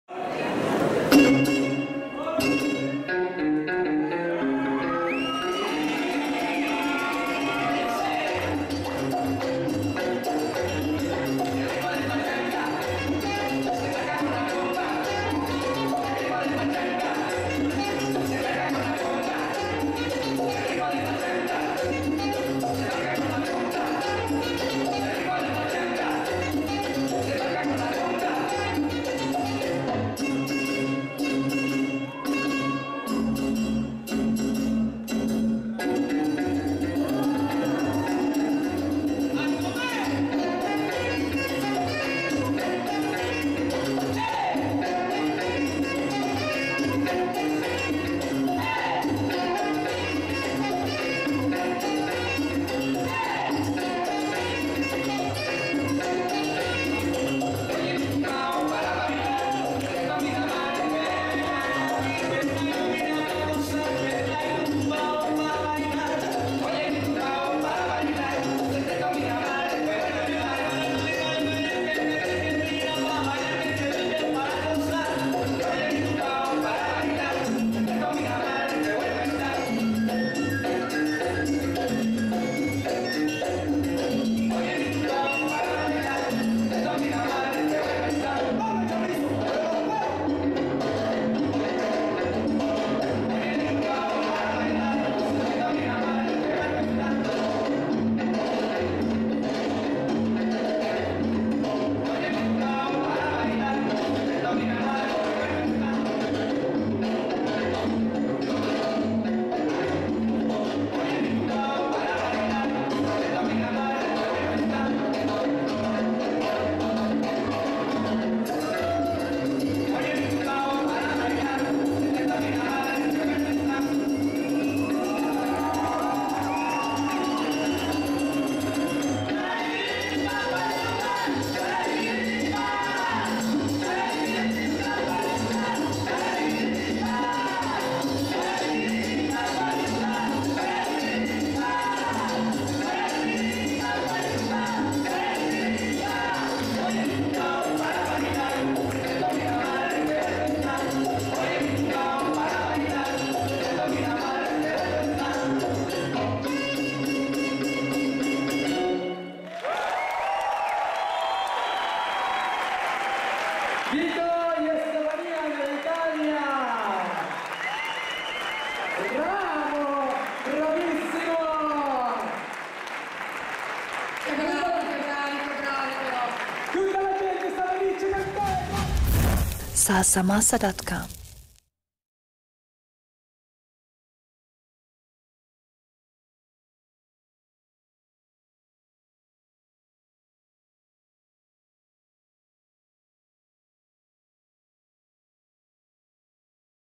VITO & STEFANIA PACHANGA SHOW | ISTANBUL DANCE FESTIVAL
VITO RANIERI & STEFANIA ERREDE PACHANGA SHOW - THE 2ND INTERNATIONAL ISTANBUL DANCE FESTIVAL.  Event Place : The Greenpark Pendik Hotel. Organization: Kemal Keskin - Artistic Director & Manager : Deniz Seven & Isabell Holy - Symcon & Ay'dan Mice. Video recording - Montage : salsamalsa.com - Follow salsamalsa